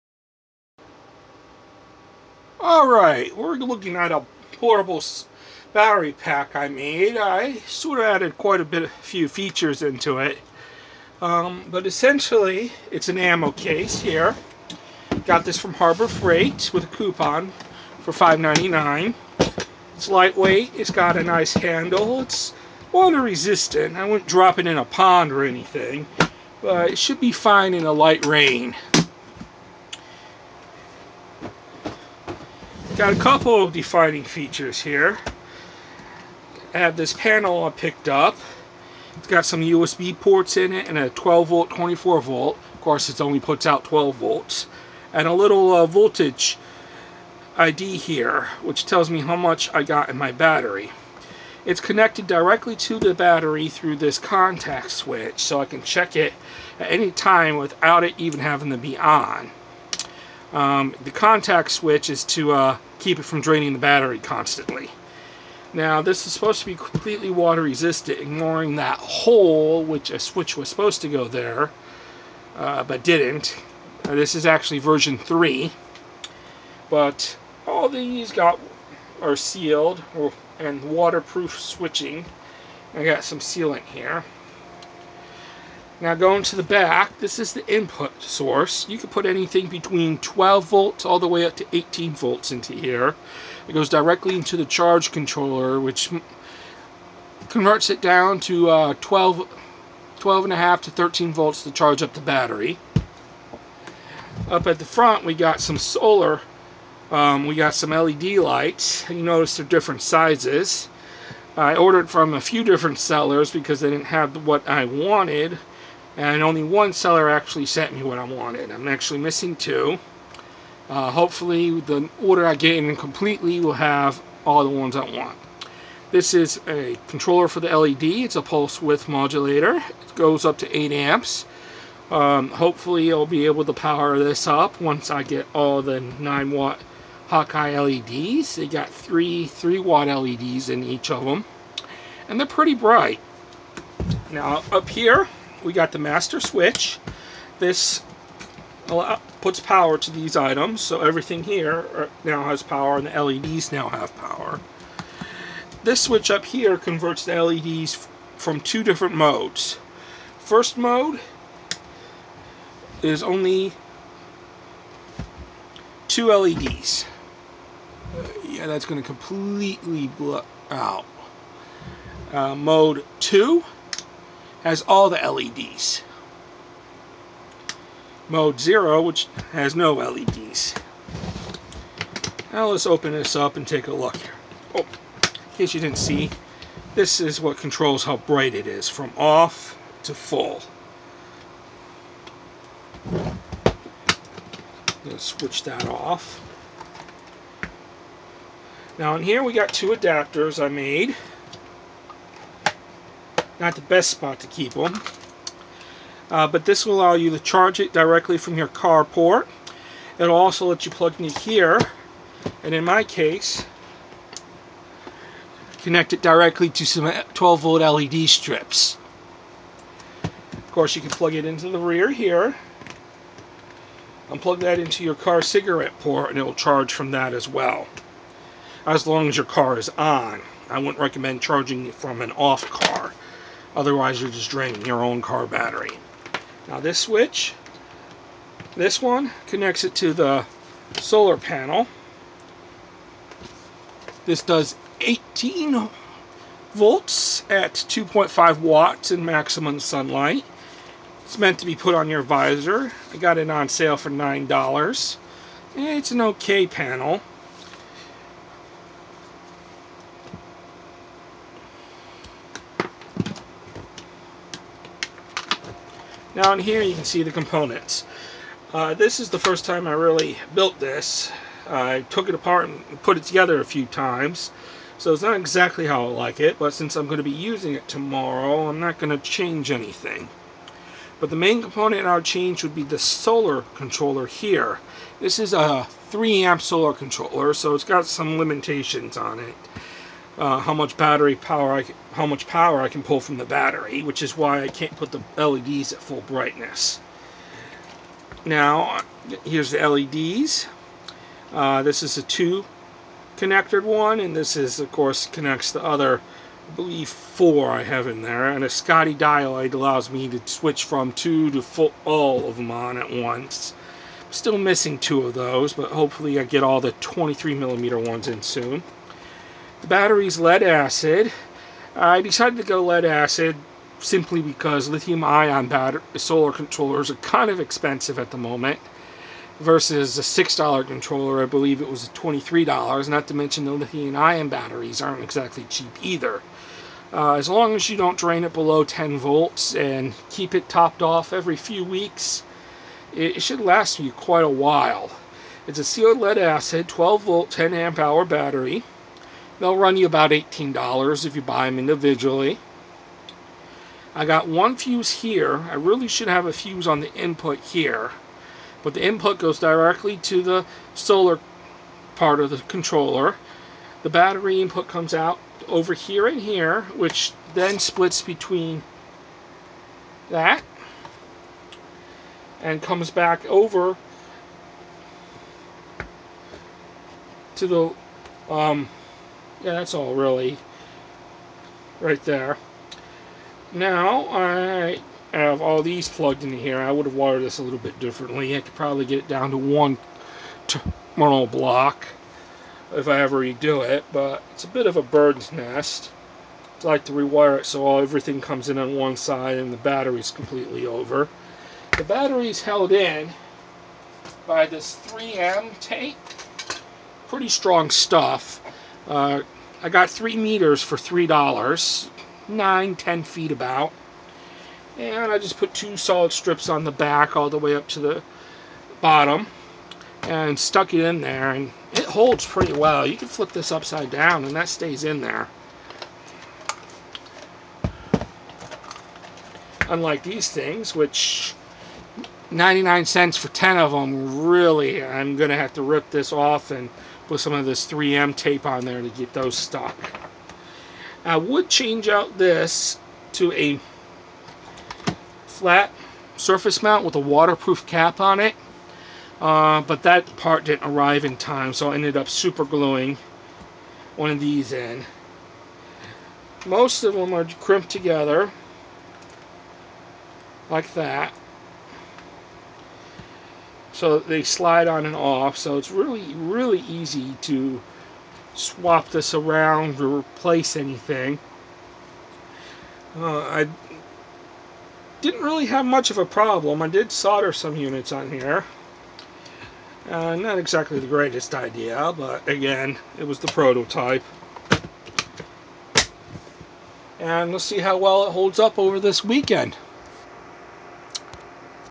DIY Portable Battery Pack 12V 10A with Solar Panel and LED
-Here is my DIY Battery pack for connecting a solar panel
-Please note I am not an electrician, all projects are at your own risk.
Here is my updated version

THIS IS A PARTS LIST FOE THE NEW VERSION ABOVE, I no longer have the original parts list since it was deleted in a google drive issue. I used different parts in the two versions.